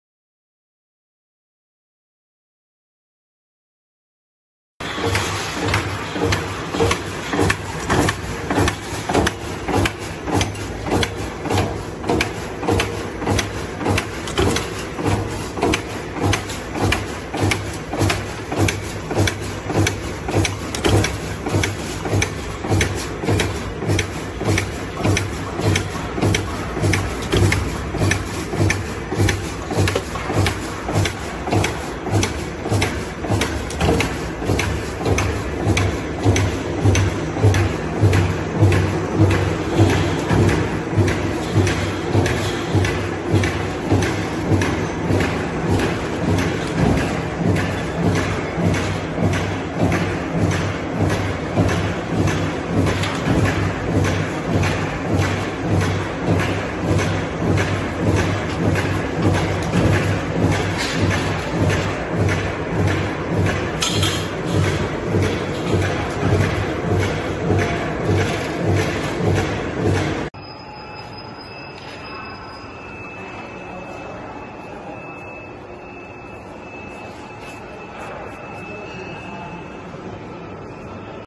JCR OPEN CELL CEILING GRILLIATO ROLL FORMING PRODUCTION LINE
Open cell ceiling grilliato roll forming production line,
More information, please contact Rachel from JCR Machine